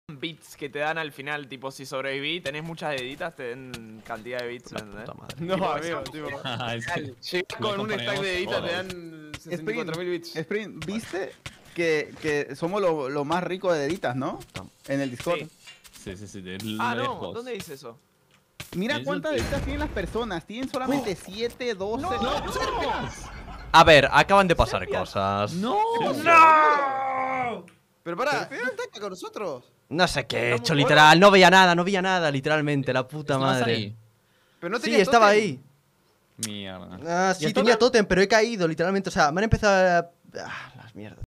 SERPIAS MUERE HORRIBLE | DEDSAFIO 2 | DIA 13
Serpias muere por caida/fuego/mobs/veneno en Dedsafio 2 XD